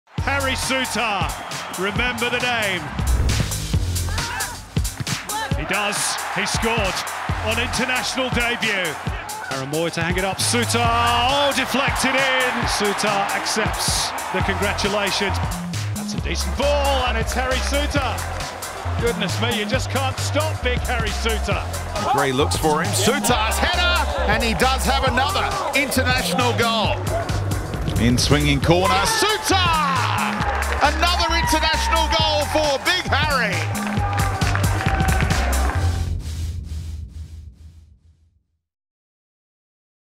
Every Harry Souttar goal for the Socceroos
Watch Harry Souttar's six goals in five games for the Socceroos!